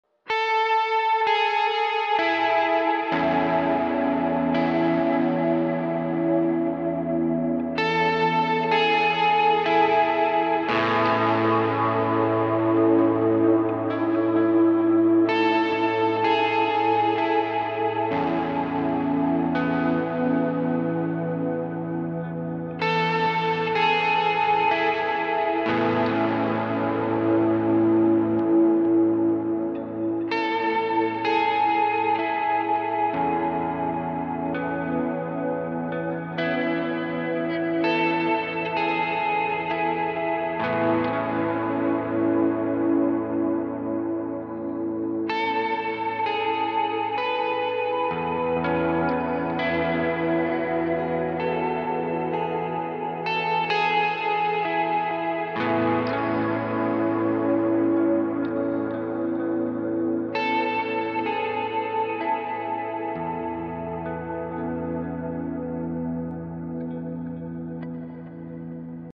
I got a new reverb and delay pedal... 2020
Here’s a brief audio sample of me utilizing some new wet effects. What do you think they are?